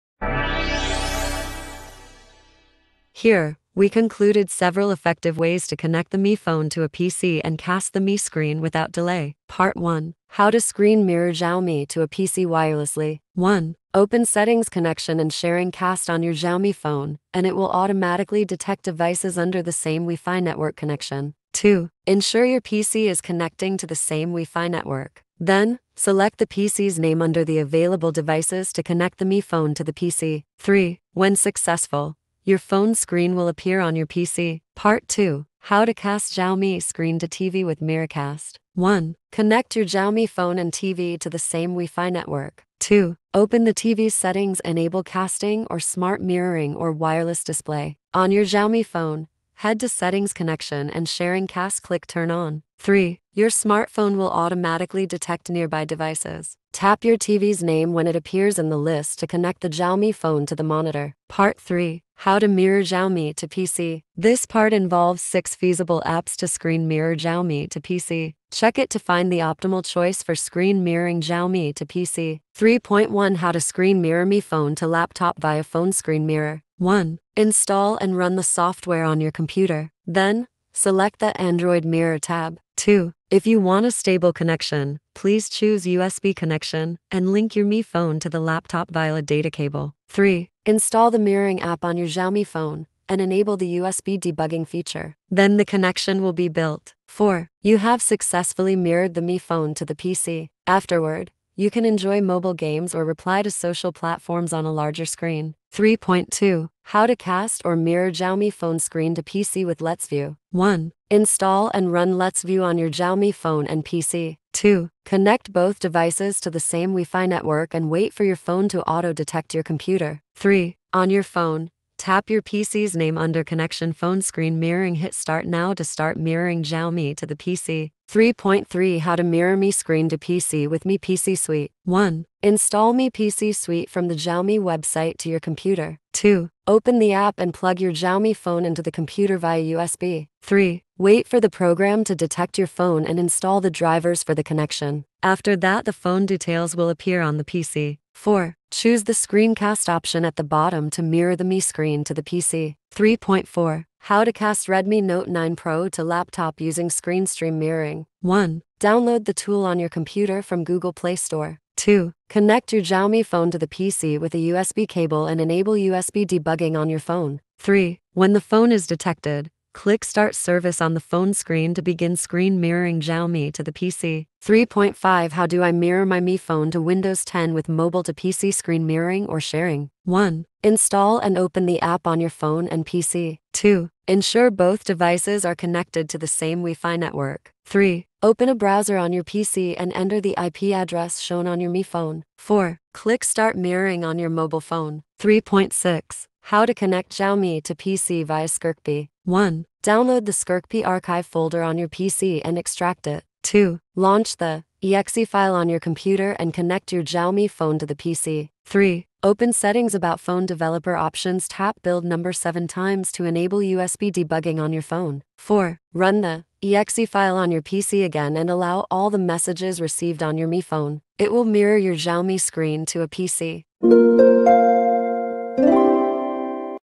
How to Screen Mirror Xiaomi to PC/TV in 8 Ways
Want to start screen mirroring Xiaomi to PC but don't know how? This video introduces 8 pragmatic ways to mirror the Mi screen to a PC or TV.

00:00 Start
00:09 Part 1. How to Screen Mirror Xiaomi to a PC Wirelessly
00:34 Part 2. How to Cast Xiaomi Screen to TV with Miracast
01:01 Part 3. How to Mirror Xiaomi to PC
01:12 3.1 How to Screen Mirror Mi Phone to Laptop via Phone Screen Mirror
01:45 3.2 How to Cast/Mirror Xiaomi Phone Screen to PC with LetsView
02:07 3.3 How to Mirror Mi Screen to PC with Mi PC Suite
02:32 3.4 How to Cast Redmi Note 9 Pro to Laptop Using Screen Stream Mirroring
02:54 3.5 How Do I Mirror My Mi Phone to Windows 10 with Mobile to PC Screen Mirroring/Sharing
03:15 3.6 How to Connect Xiaomi to PC via Scrcpy